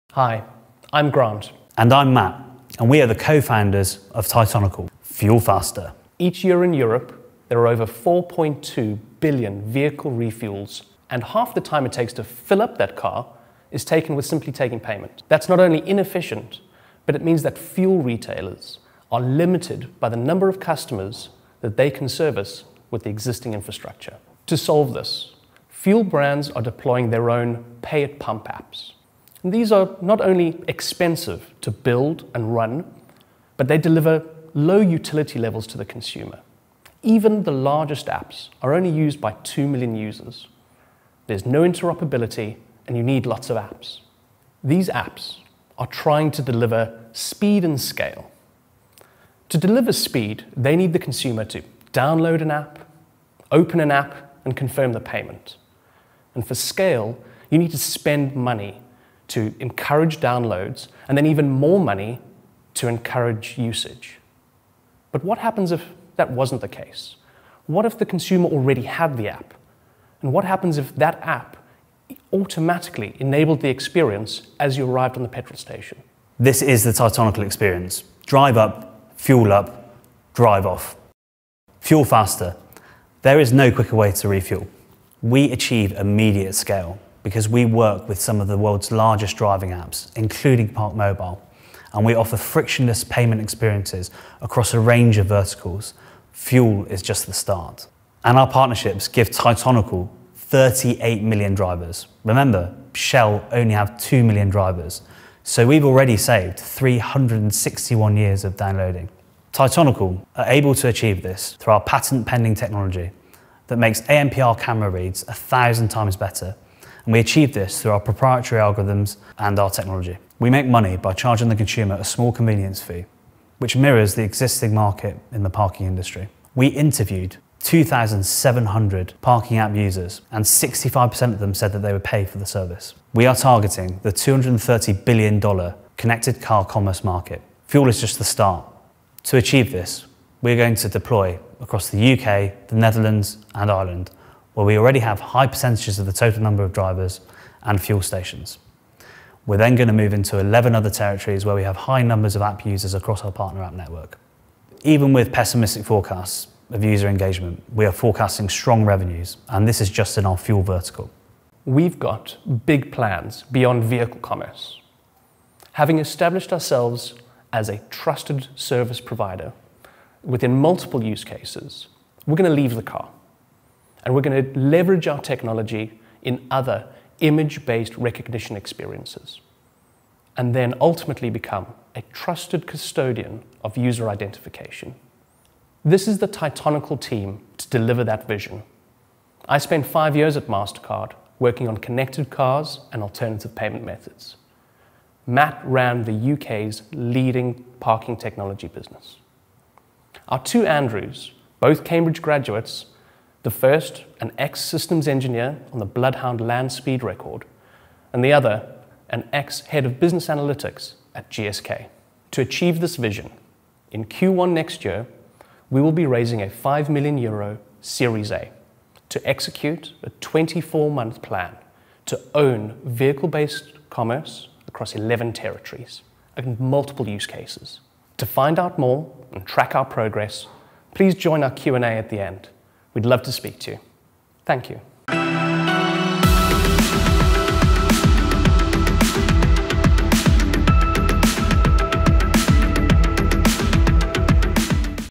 Meet Tytonical - FinTech & Cybersecurity 2020
About Tytonical: Tytonical uses a patent pending technology to identify consumers and facilitate frictionless vehicle based payments.
Problem and Opportunity: 50% percent of the time it takes to refuel a vehicle is taken with simply making the payment, and this is just the first problem in a $230Bn vehicle based payment market. Tytonical doubles the throughput for petrol stations, using existing infrastructure, and delivers the best user experience possible. Drive up, Fuel up, Drive off - Fuel Faster.